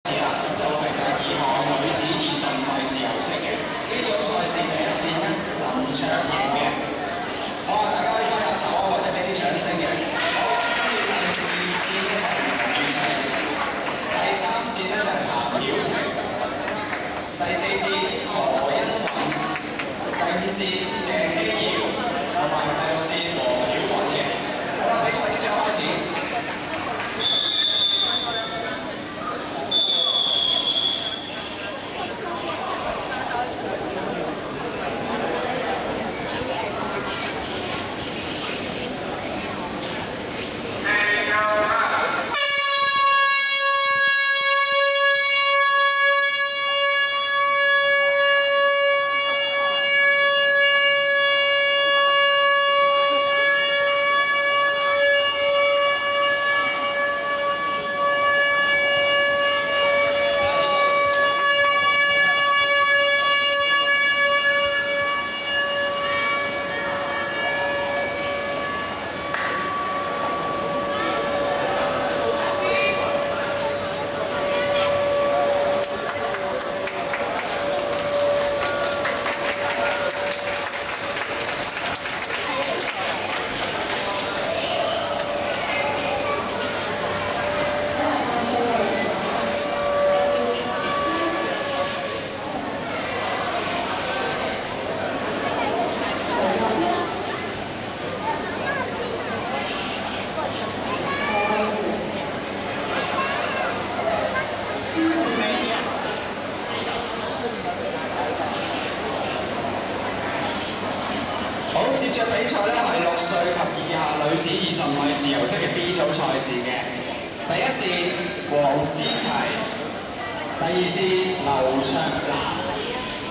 00051.MTS
2010年9月22日小小海豚水運會25m自由式6歲或以下第1組